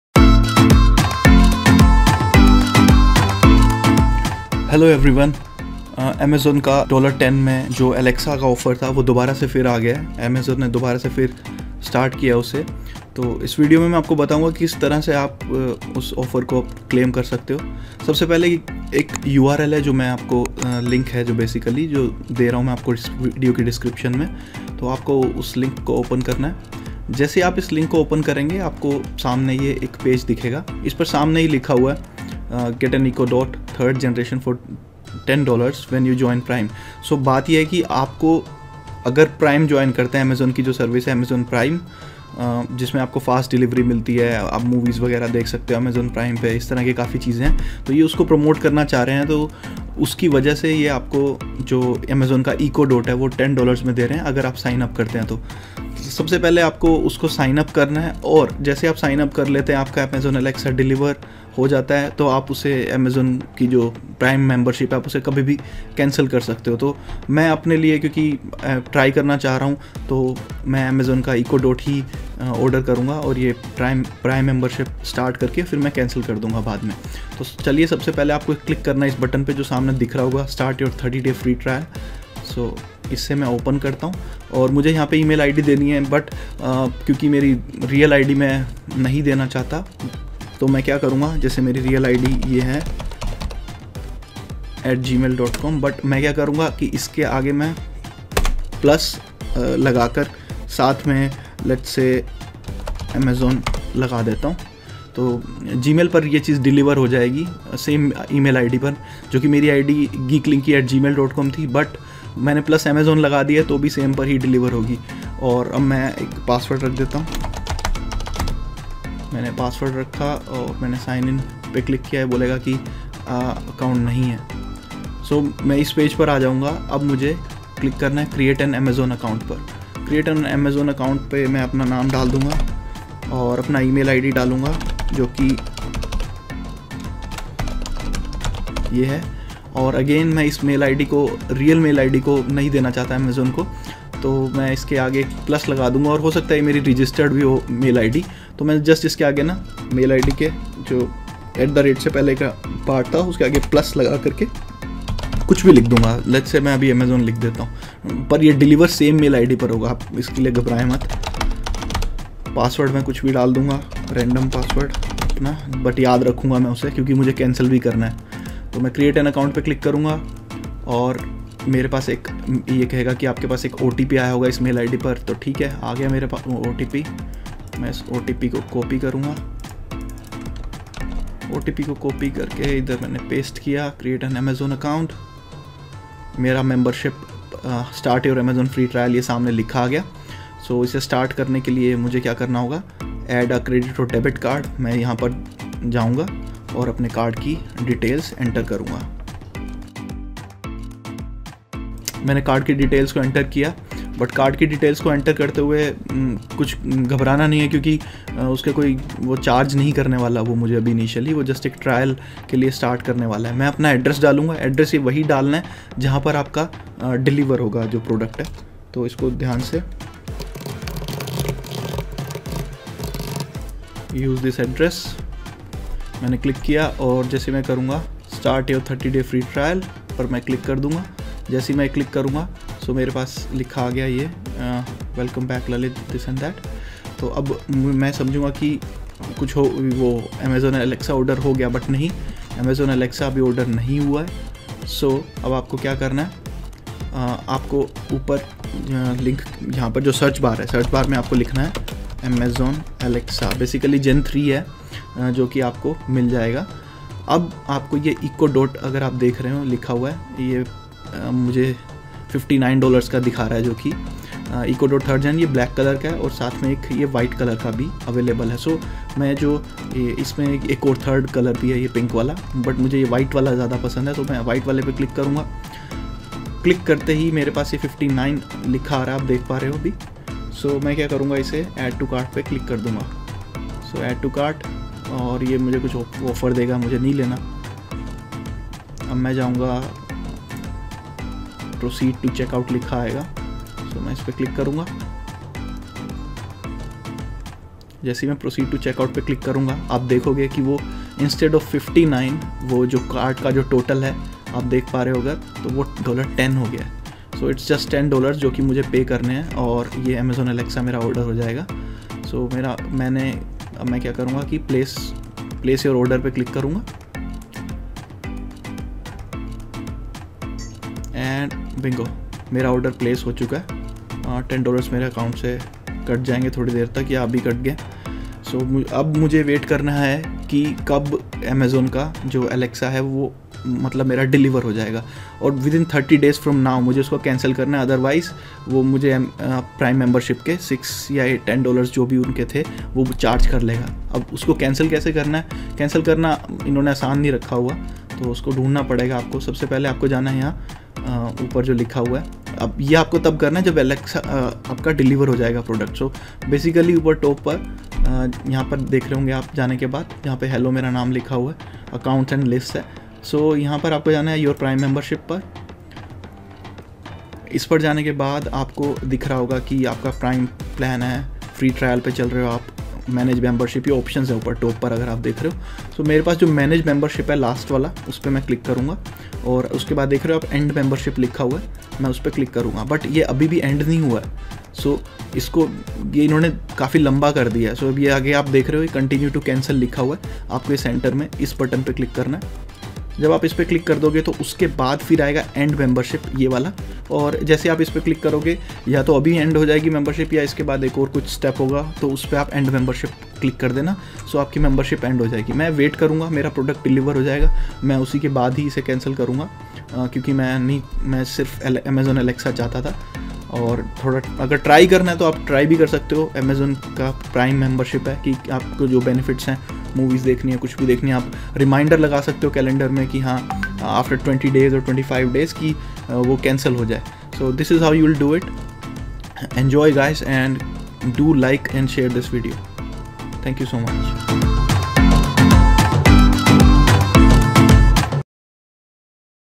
Get Amazon Alexa Echo Dot Gen 3 in $10 only. Here is the Amazon Trick (Australia Only) - Hindi
This video is a step by step video how you can get the amazing Amazon Alexa Echo Dot 3rd Generation in just $10. (Explanation in Hindi)

Please note that if you forget to cancel your subscription, i am not at all responsible for the subscription renewal.